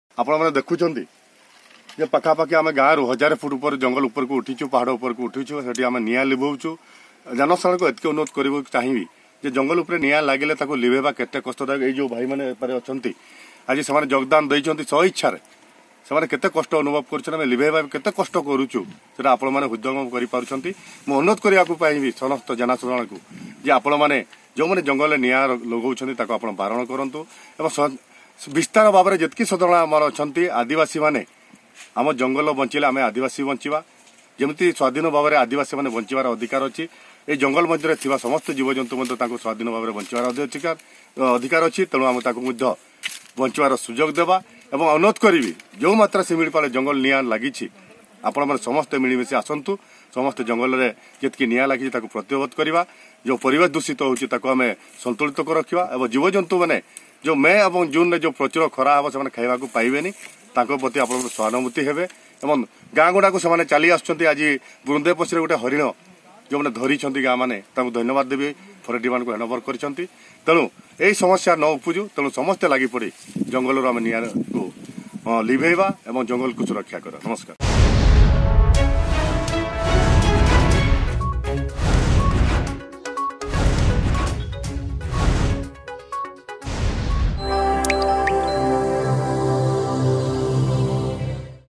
ଶିମିଳିପାଳ ରେ ନିଆଁ ଲିଭାଇବାକୁ ଅନୁରୋଧ...
ଚକ୍ରଧର ହେମ୍ବ୍ରମ, ପୂର୍ବତନ ବନ୍ୟ ଜୀବନ ୱାଡେନ,ସାମାଜିକ କାର୍ଯ୍ୟକର୍ତ୍ତା ଟିମ ସହିତ ଶିମିଳିପାଳ ରେ ନିଆଁ ଲିଭାଇବାକୁ ଅନୁରୋଧ କରୁଛନ୍ତି...